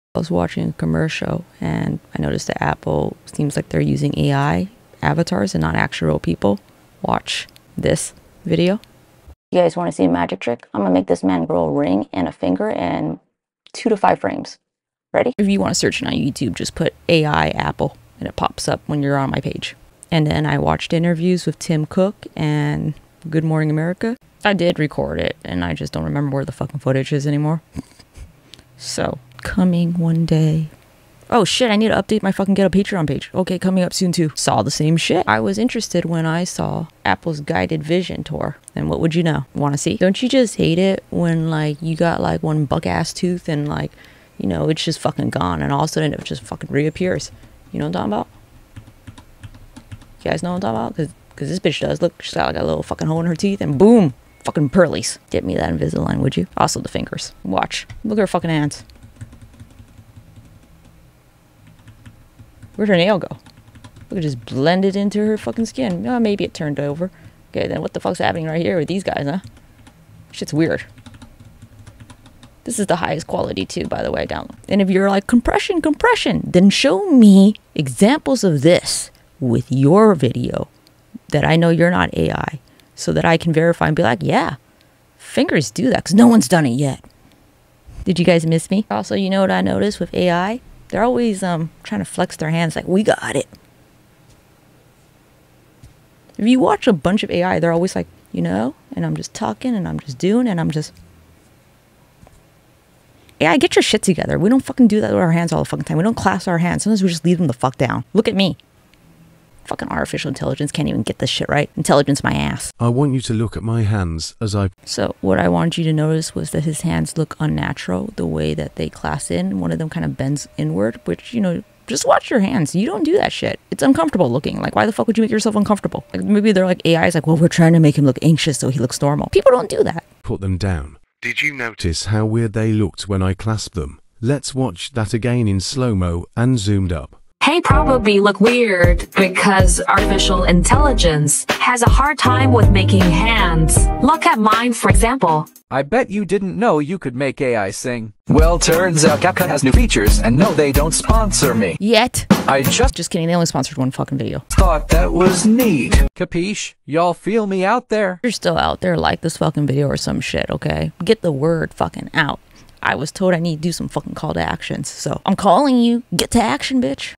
apple why are you using ai instead of people again in your commercials and tutorials?
Don't you know that's deceiving!? tech dafuq is happening here.